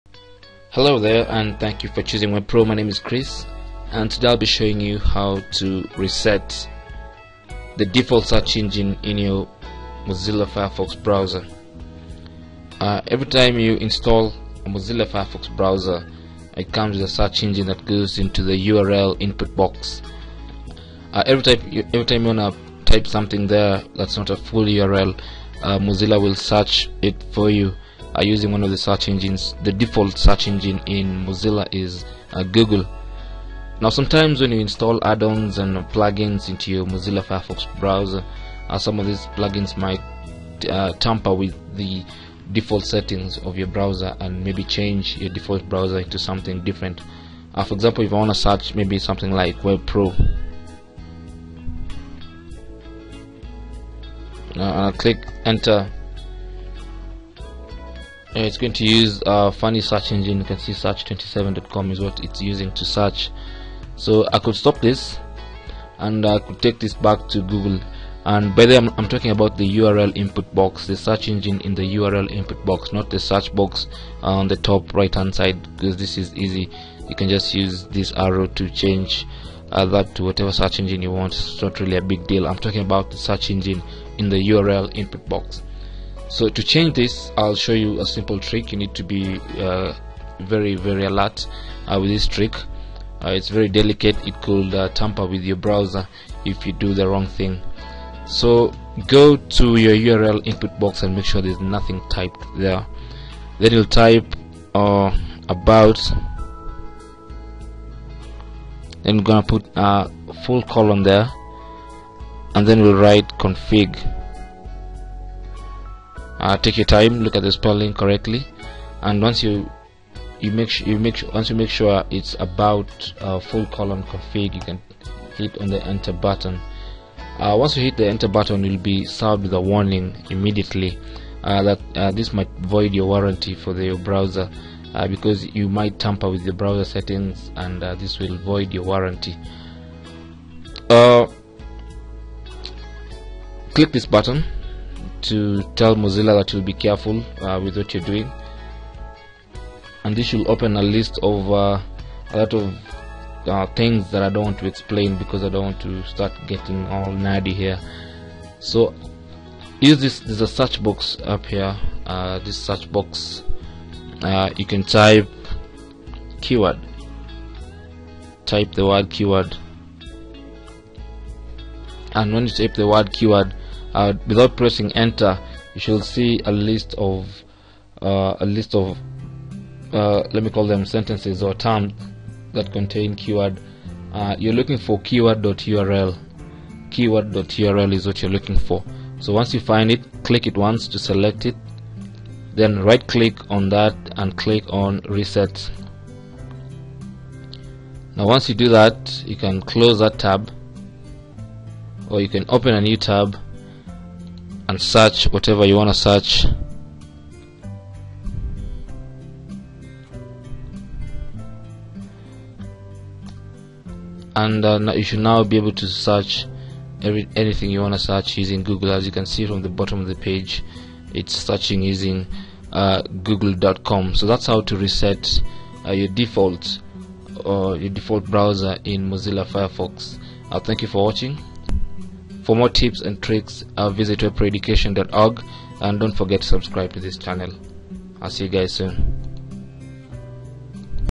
Reset The Firefox Default Search Engine
This video will show you ho to reset the default search engine for Firefox back to Google. change default search engine firefox.
When you install Firefox, it has a default search engine which is the firefox search engine powered by Google.

How To change default search engine firefox
Addons and malware will attempt to change your browser's default search engine to something different. This video tutorial shows you a step by step process of resetting the default search engine that came with your Firefox browser.